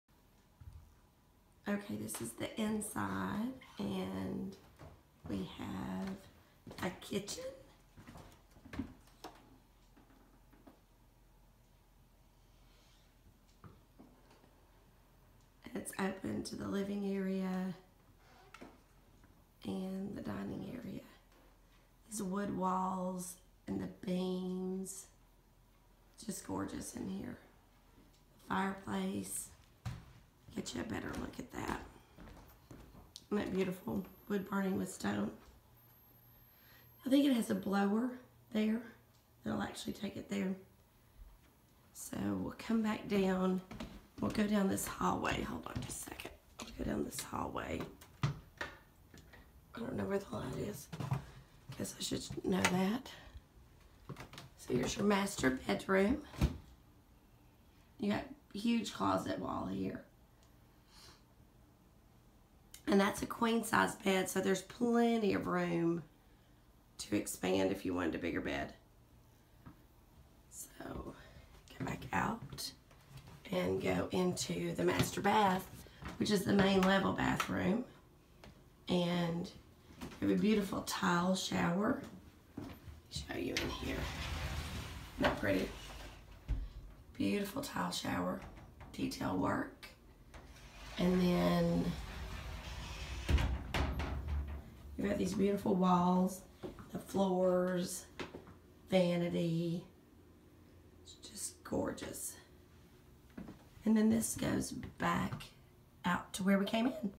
1287 cove point lafollette. Norris lake home for sale
Norris lakefront home for sale. This home is perfect and offers a great position on the lake. Just minutes to several marinas.